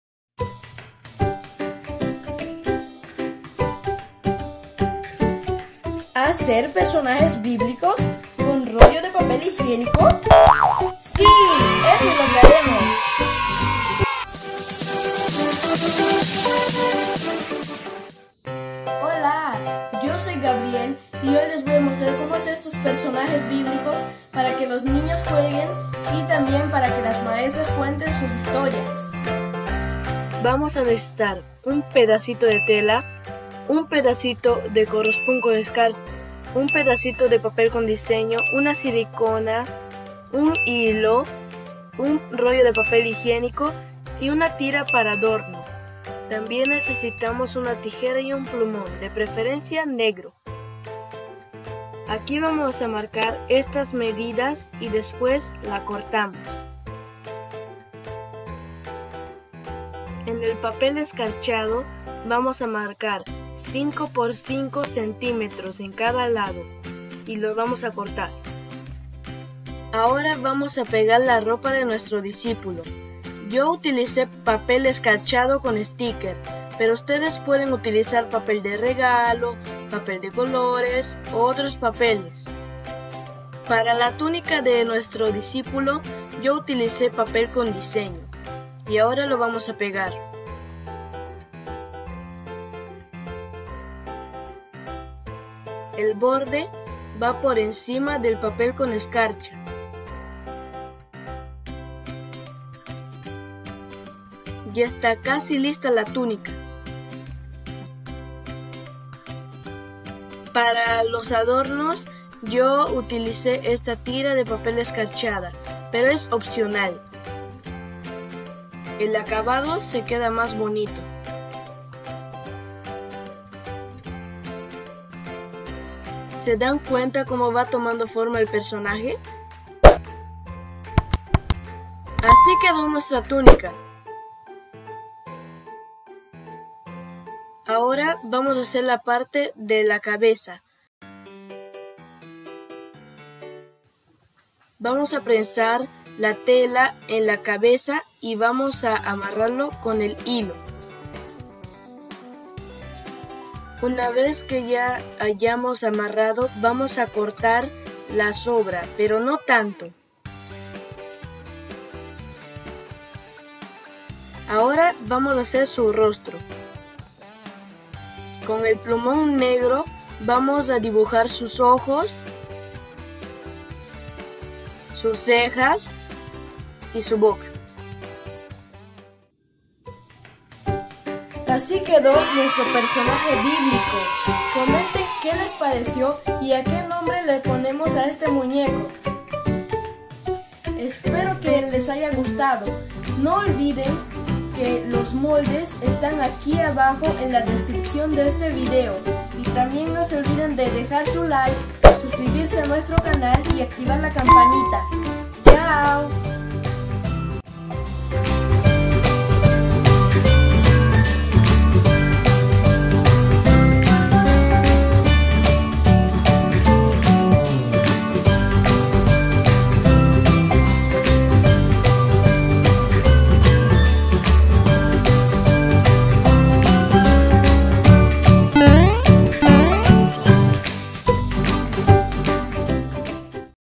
COMO HACER MUÑECOS BÍBLICOS CON ROLLO DE PAPEL HIGIÉNICO-RECICLAJE
Las medidas están disponibles para impresión en nuestra página:
MATERIALES NECESARIOS:
Rollo de papel higiénico
Pegamento
Plumón negro delgado
Papel de colores o de regalo
tijera
1 pedacito de tela
1 pedazo de 20 cm de liga o hijo